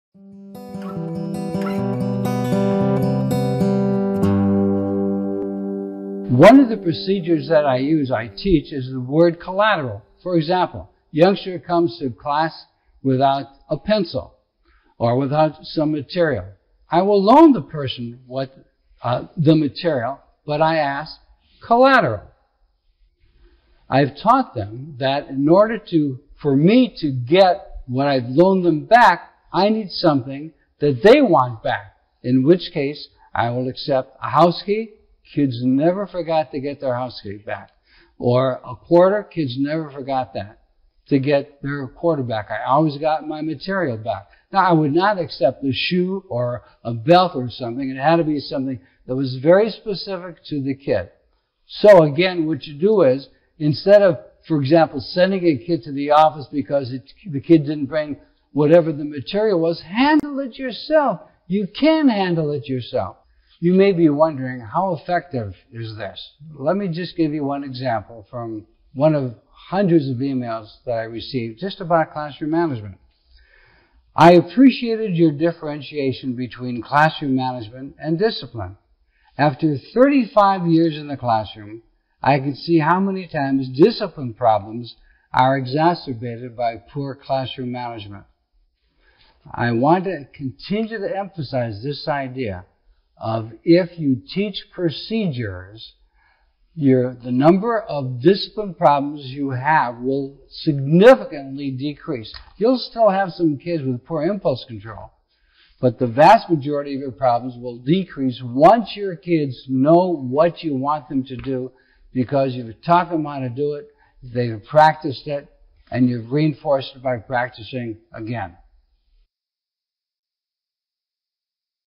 Module 7 - Using Collateral
This segment shows how to use collateral when students come to class without required materials. The segment concludes with a testimonial from a teacher who finally understood the difference between classroom management and discipline and how many times discipline problems are prompted by poor classroom management.

To have Dr. Marvin Marshall speak at your next event,